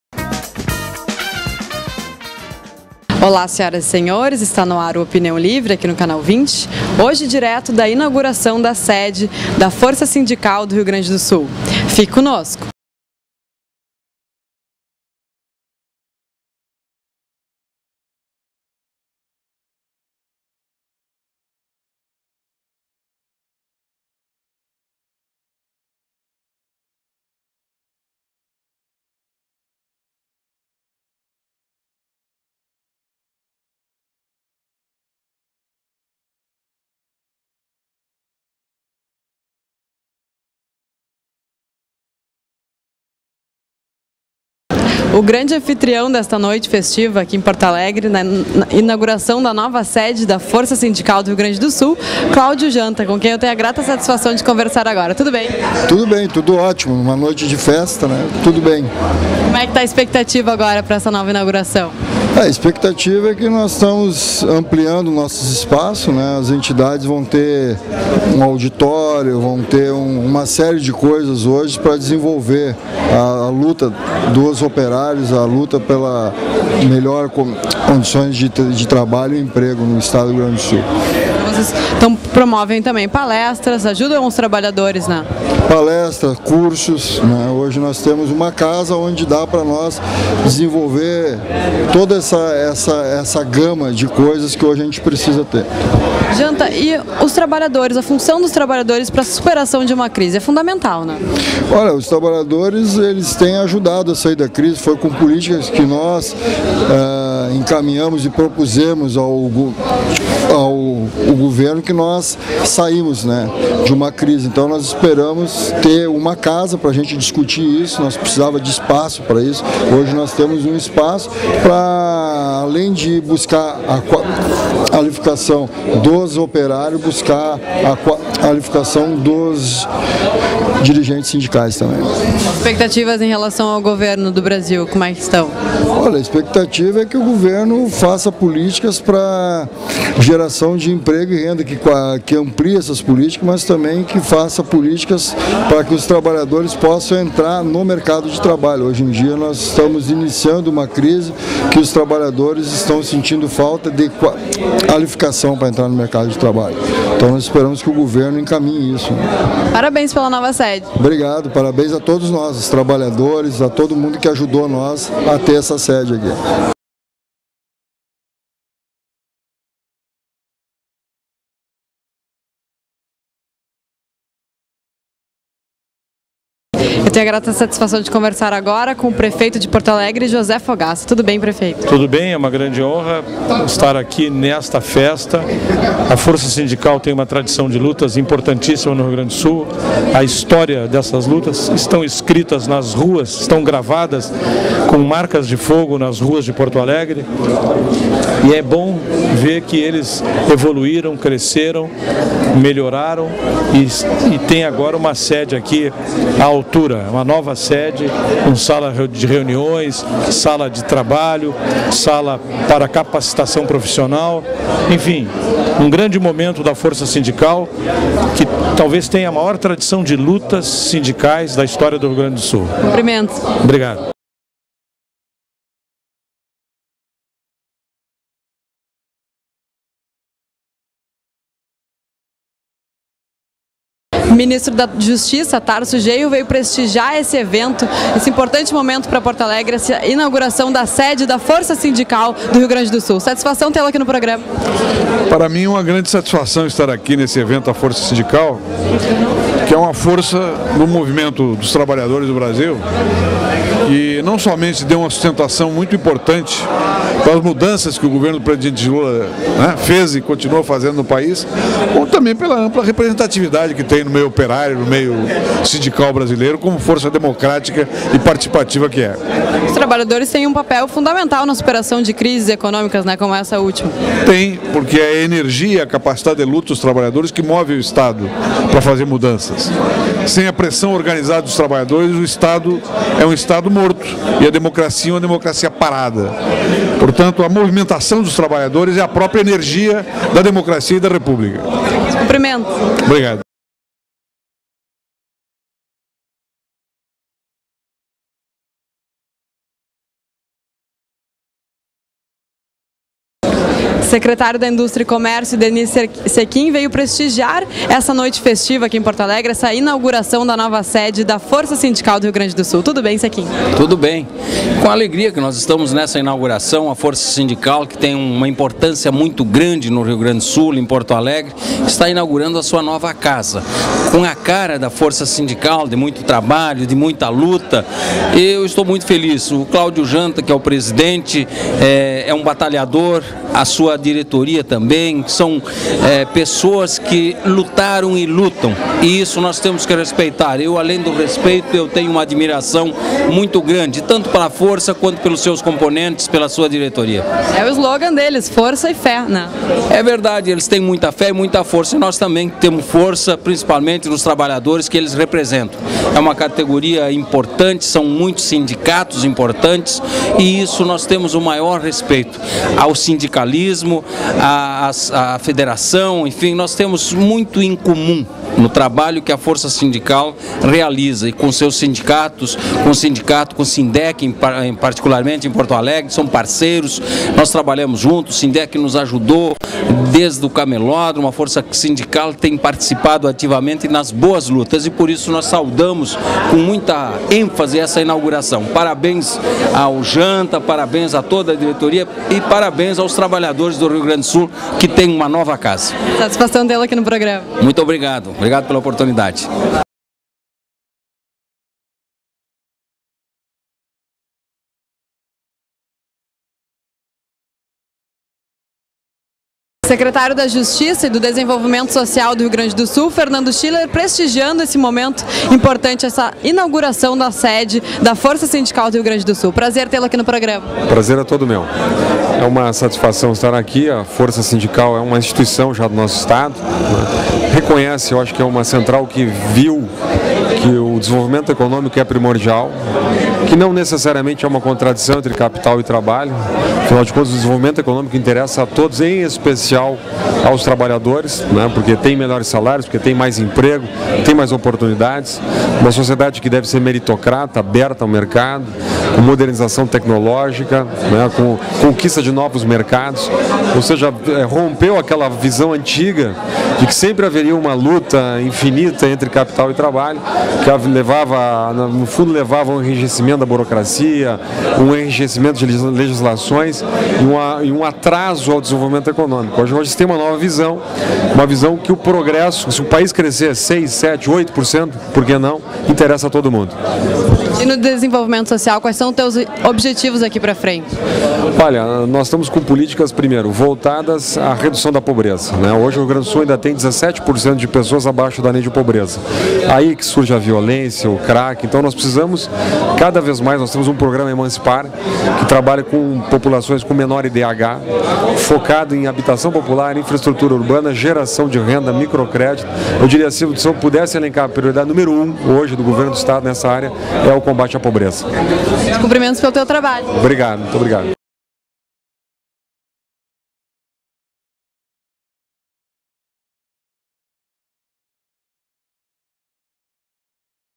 Opinião Livre - Forca Sindical inaugura nova sede I
Gabriella Bordasch entrevista:

- Cláudio Janta - Presidente Força Sindical - RS
- José Fogaça - Prefeito de Porto Alegre
- Tarso Genro - Ministro da Justiça
- Idenir Cecchin - Secretário Industria e Comércio - POA
- Fernando Schüler - Secretário Justiça e Desenvolvimento - RS